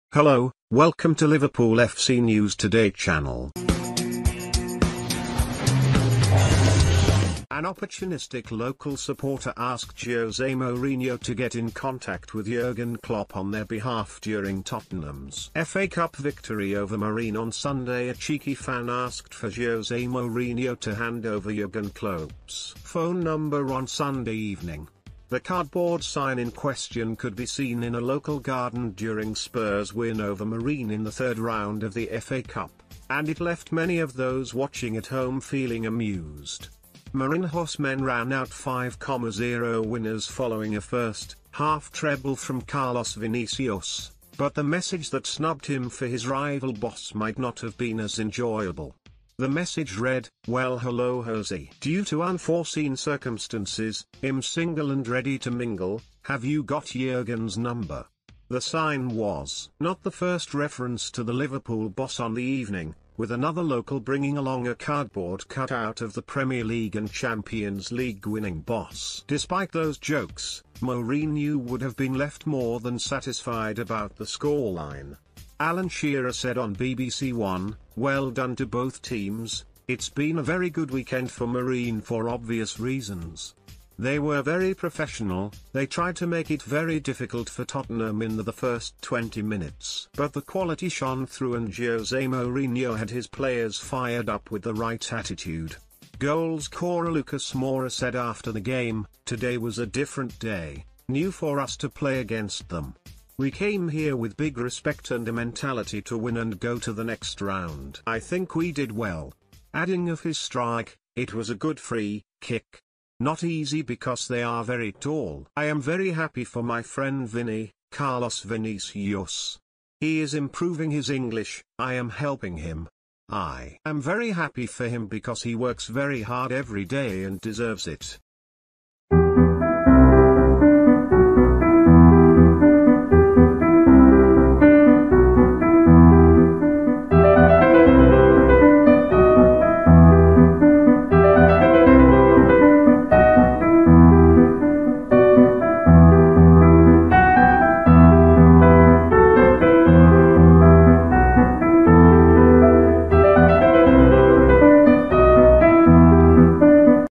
Cheeky Marine fan tells Jose Mourinho they're single - and asks for Jurgen Klopp's number - new...
Cheeky Marine fan tells Jose Mourinho they're single - and asks for Jurgen Klopp's number - news today

An opportunistic local supporter asked Jose Mourinho to get in contact with Jurgen Klopp on their behalf during Tottenham’s FA Cup victory over Marine on Sunday A cheeky fan asked for Jose Mourinho to hand over Jurgen Klopp’s phone number on Sunday evening. The cardboard sign in question could b...